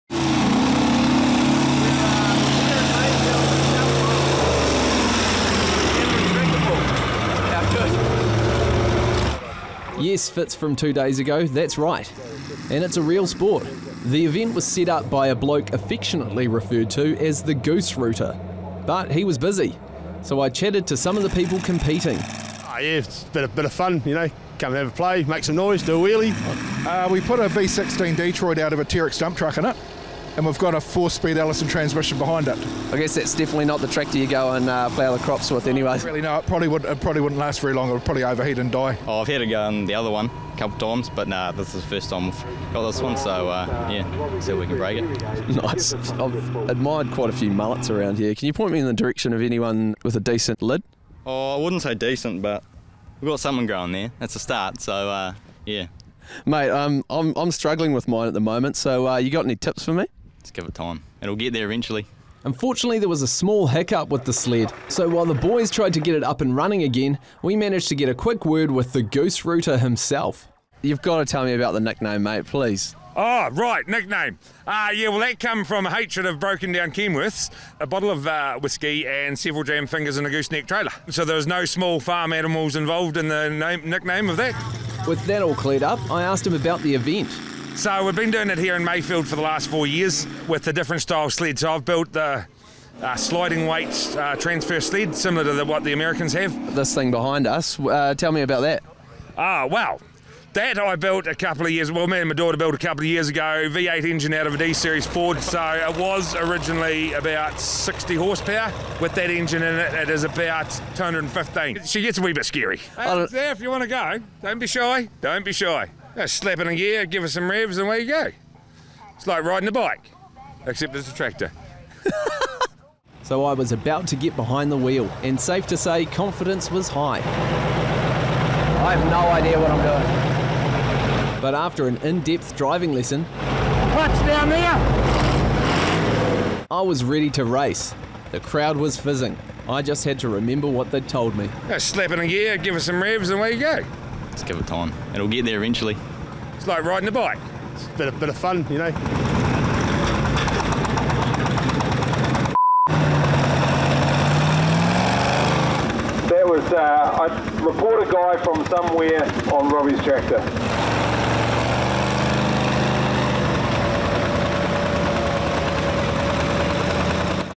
The Gooserooter From Geraldine, Tractor Pull Wheels Week Plus Event March 14th 2022
Canterbury Tractor Pull Wheels Week Plus Event
Thanks to sky tv and the crowd goes wild for the filming and editing